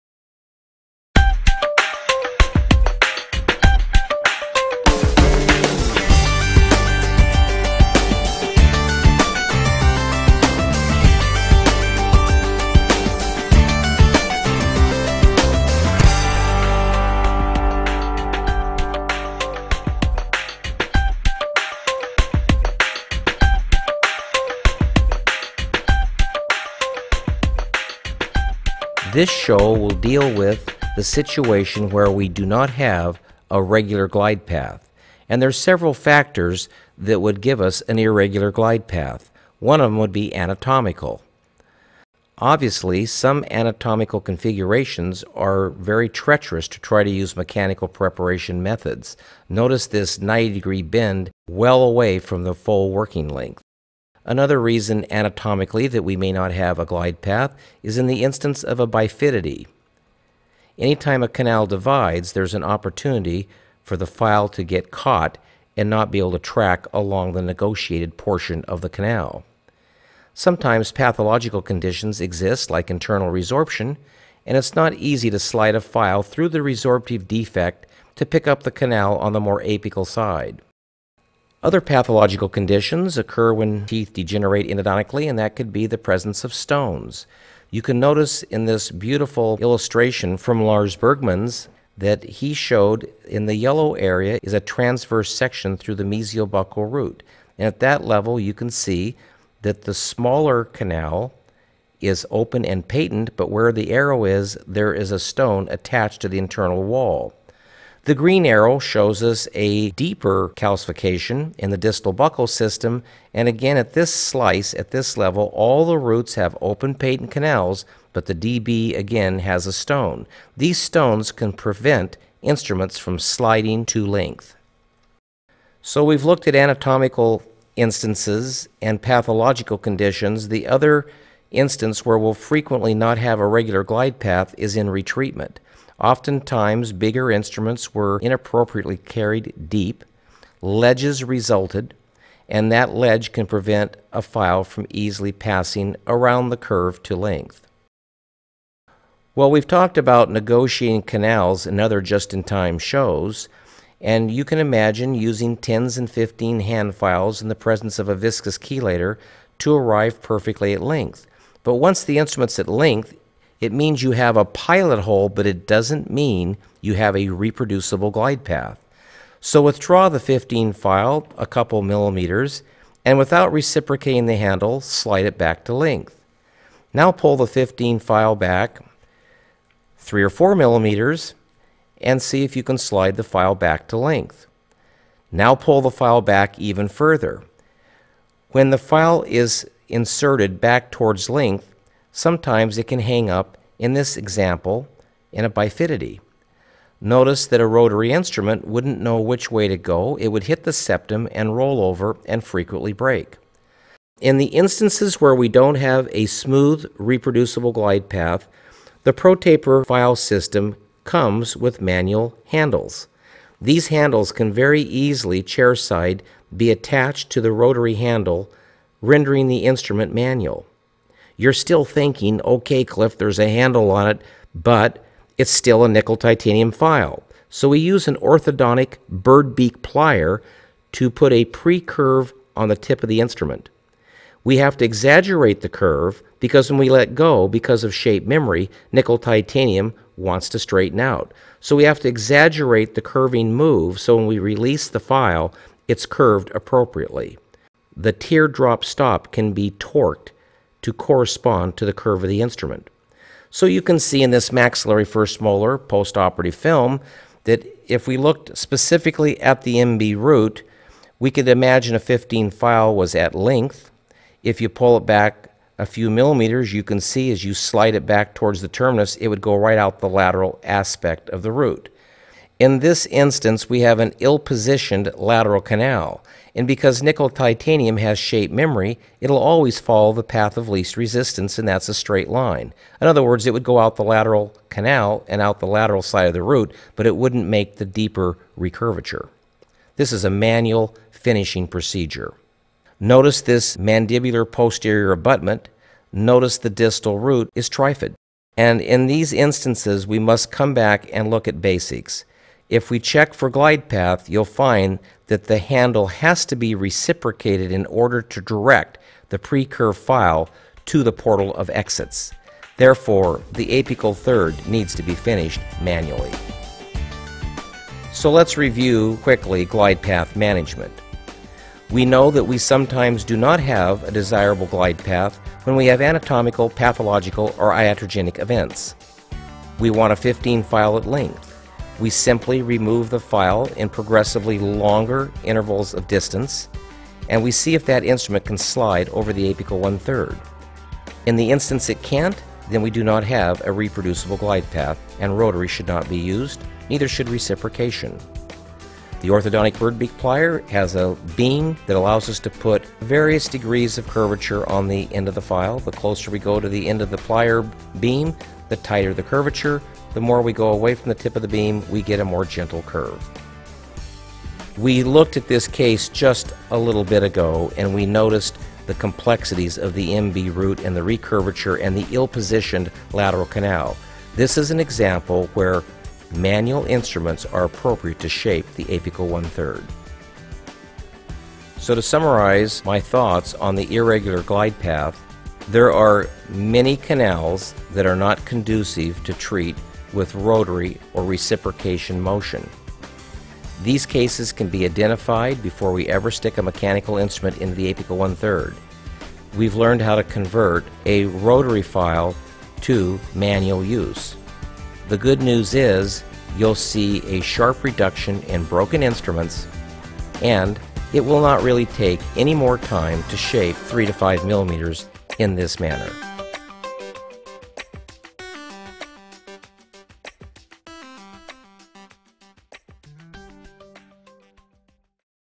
Shape Clean Pack: Glide Path Management - Irregular Glidepath: Dr. Ruddle Advanced Endodontics
This show will identify the three causes that lead to an irregular glide path.  Clinically, you will be shown exactly how to negotiate canals and then determine with 100% confidence, if you do or do not have a smooth, reproducible glide path.  Importantly, how to pre-curve small-sized hand files and negotiate abruptly curved canals will be reviewed.  Finally, you will be excited to see how to pre-curve NiTi shaping files so you can more safely prepare canals that exhibit an irregular glide path.
this show will deal with the situation where we do not have a regular glide path and there are several factors that would give us an irregular glide path one of them would be anatomical
obviously some anatomical configurations are very treacherous to try to use mechanical preparation methods notice this 90-degree bend well away from the full working length another reason anatomically that we may not have a glide path is in the instance of a bifida T any time a canal divides there's an opportunity for the file to get caught and not be able to track along the negotiated portion of the canal sometimes pathological conditions exist like internal resorption and it's not easy to slide a file through the resort of defect to pick up the canal on the more apical side other pathological conditions occur when teeth degenerating ionically and that could be the presence of stones you can notice in this beautiful illustration from Lars Bergman's that he showed in the yellow area is a transverse section through the mesial-buccal root and at that level you can see that the smaller canal is open and paitent but where the arrow is there is a stone attached to the internal wall the green arrow shows us a deeper calcification in the distal buccal system and again at this slice at this level all the roots have opened paetynn canals but the DB again has a stone these stones can prevent instruments from sliding to length so we've looked at anatomical instances and pathological conditions the other instance where we'll frequently not have a regular glide path is in retreatment often times bigger instruments were inappropriately carried deep ledges resulted and that ledge can prevent a file from easily passing around the curve to length well we've talked about negotiating canals and other just-in-time shows and you can imagine using tens and 15 hand files in the presence of a viscous key later to arrive perfectly at length but once the instruments at length it means you have a pilot hole but it doesn't mean you have a reproducible glide path so withdraw the 15 file a couple millimeters and without reciprocating the handle slide it back to length now pull the 15 file back three or four millimeters and see if you can slide the file back to length now pull the file back even further when the file is inserted back towards length sometimes it can hang up in this example and a bifida T notice that a rotary instrument wouldn't know which way to go it would hit the septum and roll over and frequently break in the instances where we don't have a smooth reproducible glide path the pro taper file system comes with manual handles these handles can very easily chairside be attached to the rotary handle rendering the instrument manual you're still thinking okay cliff there's a handle on it but it's still a nickel titanium file so we use an orthodontic bird beak plier to put a pre curve on the tip of the instrument we have to exaggerate the curve because when we let go because of shape-memory nickel titanium wants to straighten out so we have to exaggerate the curving move so when we release the file it's curved appropriately the teardrop stop can be torqued to correspond to the curve of the instrument so you can see in this maxillary first molar post-operative film that if we looked specifically at the MB root we could imagine a 15 file was at length if you pull it back a few millimeters you can see as you slide it back towards the terminus it would go right out the lateral aspect of the root in this instance we have an ill positioned lateral canal and because nickel titanium has shape memory it'll always follow the path of least resistance and that's straightline in other words it would go out the lateral canal and out the lateral side of the route but it wouldn't make the deeper recurvature this is a manual finishing procedure notice this mandibular posterior abutment noticed the distal route is Triffids and in these instances we must come back and look